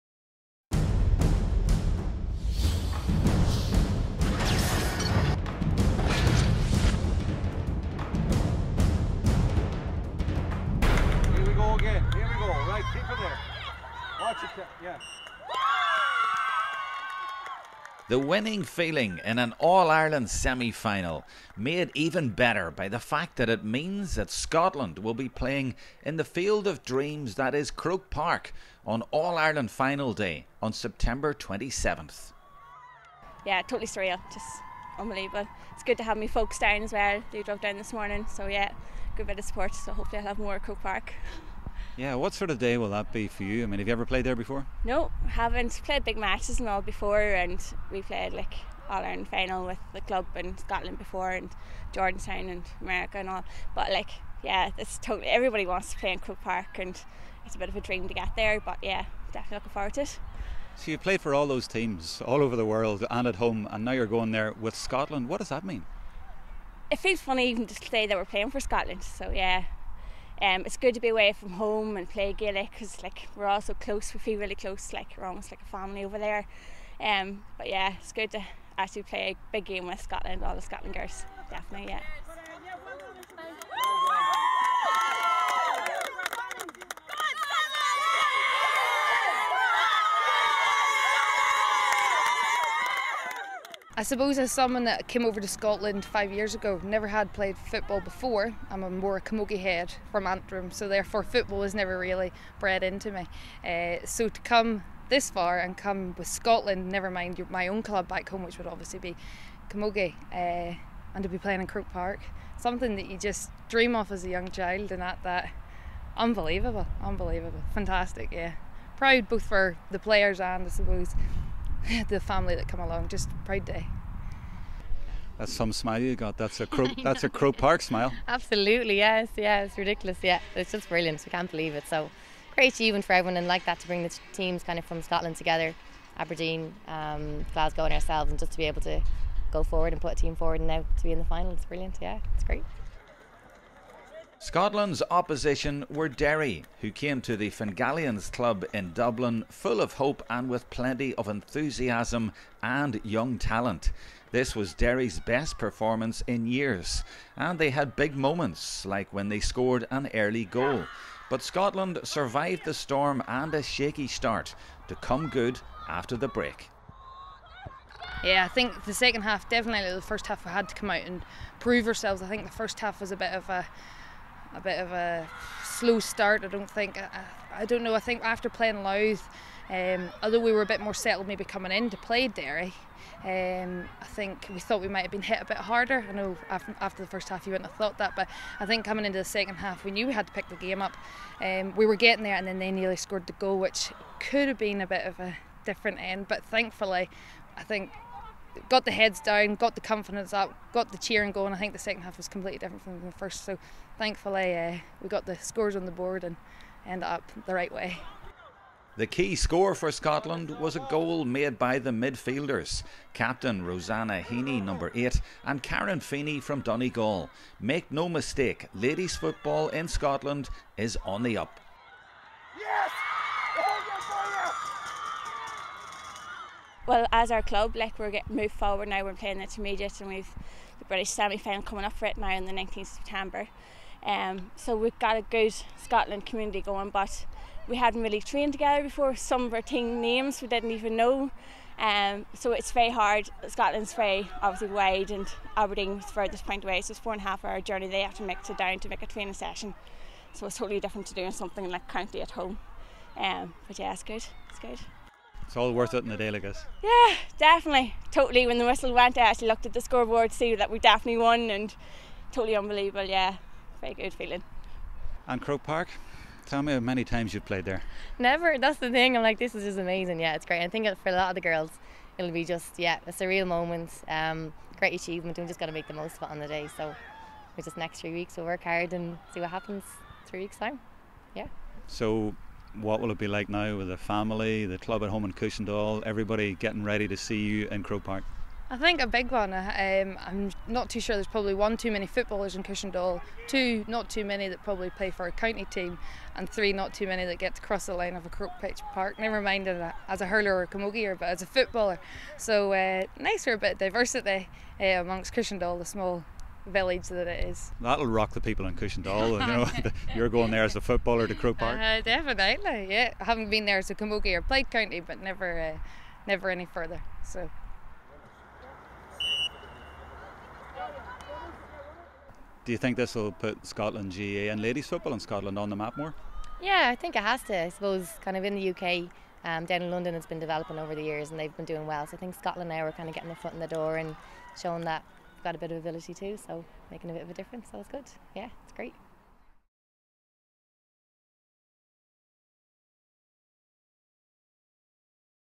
‘Proud day’ for Scotland Ladies
See a report from an historic day for Scotland GAA when their Ladies Gaelic Football team beat Derry to qualify for the 2015 TG4 All Ireland Junior Final in Croke Park on Sunday, September 27.

‘It’s surreal’, says Tyrone-born Shauna McWilliams. ‘It’s a dream to get there. It feels funny to even say we’re playing for Scotland but the girls from Aberdeen, Glasgow and Edinburgh have shown that we have ability’.

Former Cushendall and Antrim camogie player Bronagh Heggarty only took up football in Scotland and says the semi-final win is ‘unbelievable.. a proud day for the players and our families’.

Scotland will now play Louth in the All Ireland Final.

Video by Jerome Quinn for the LGFA.